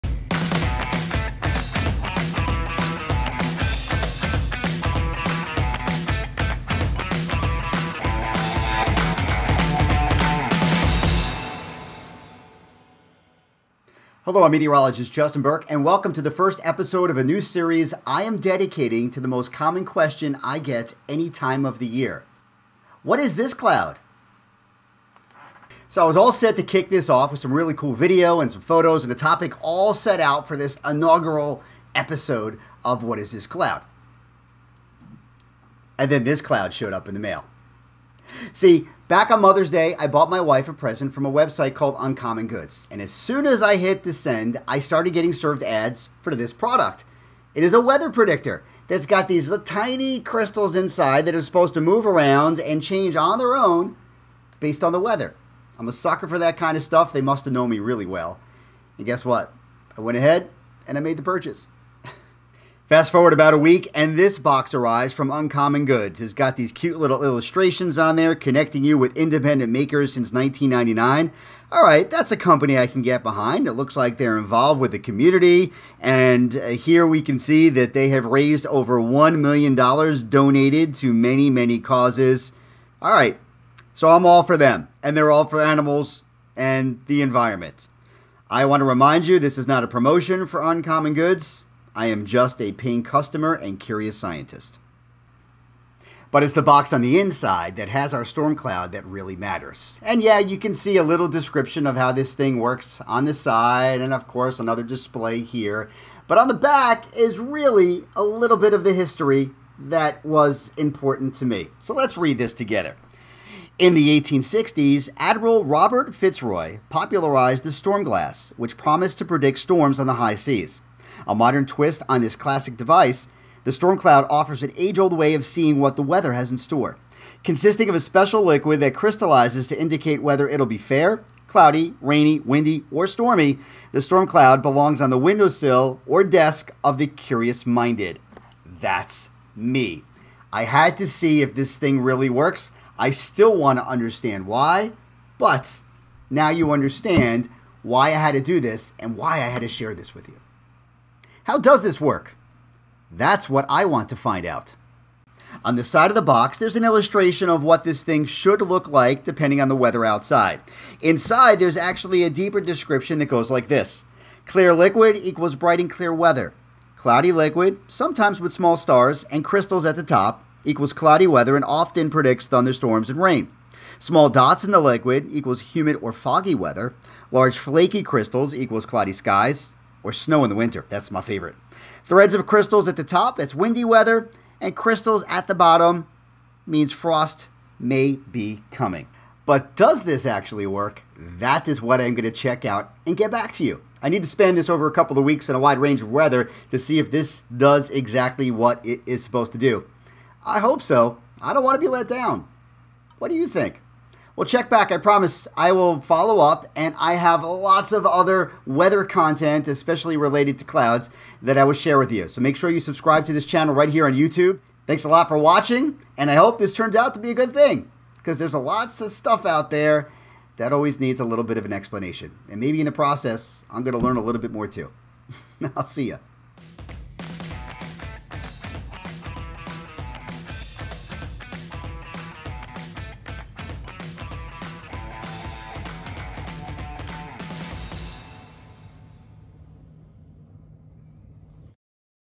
What is this cloud: Storm Cloud Predicting Instrument
Welcome to a new series based on the most common question I get asked all year: What is this cloud?
But I had to start with this Storm Cloud: Forecast from Uncommon Goods since it just arrived in the mail.
Why is it and what does it do?  Let's check it out. I will follow up to see if this actually works before I can recommend this to you.